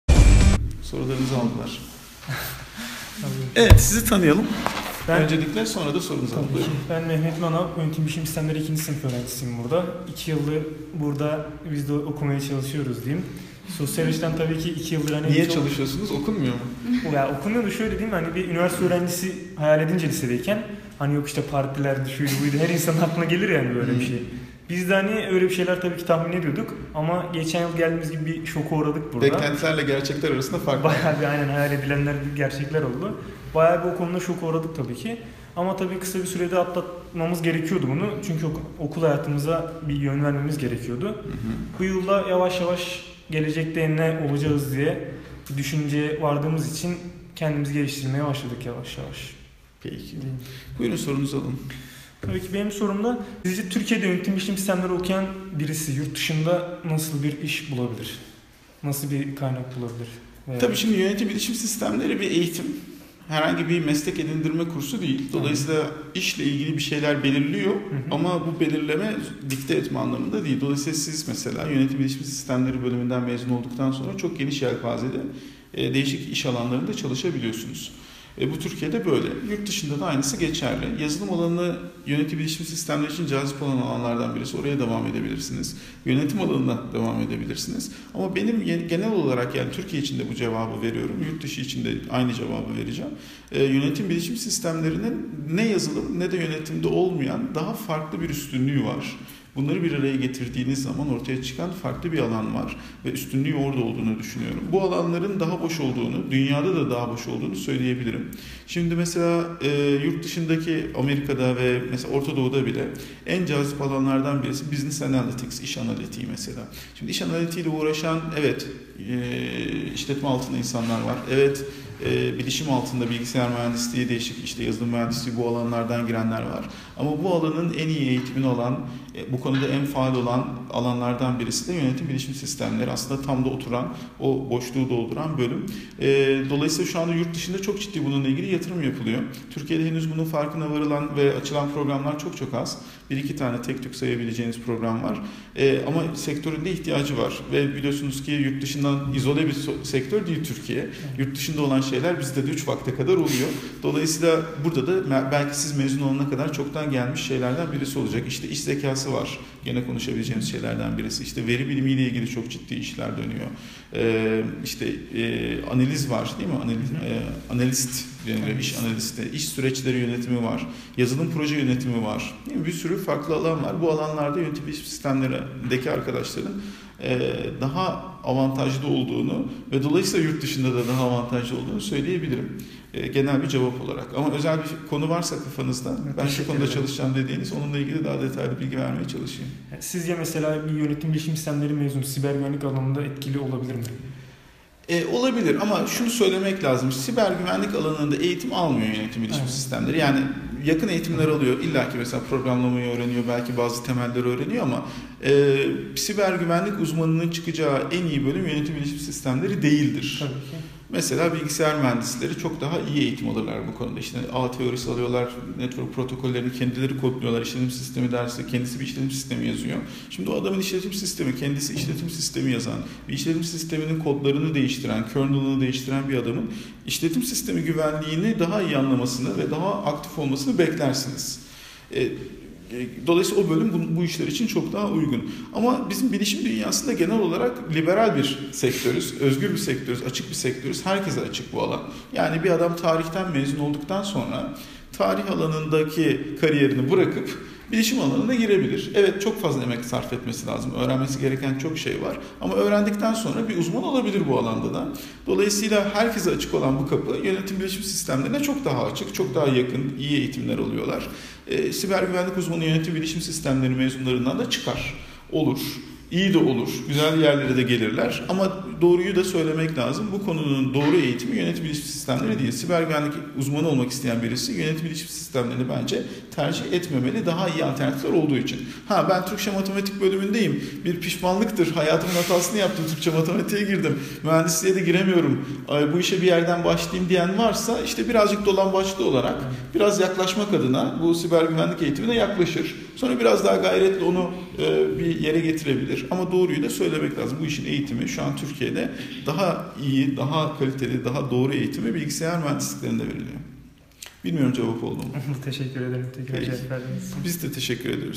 YBS Çalışma Alanı ve Siber Güvenlik
Uludağ Üniversitesi Yönetim Bilişim Sistemleri 2. sınıf öğrencisi Mehmet Önal'ın sorularını cevaplamaya çalışıyoruz.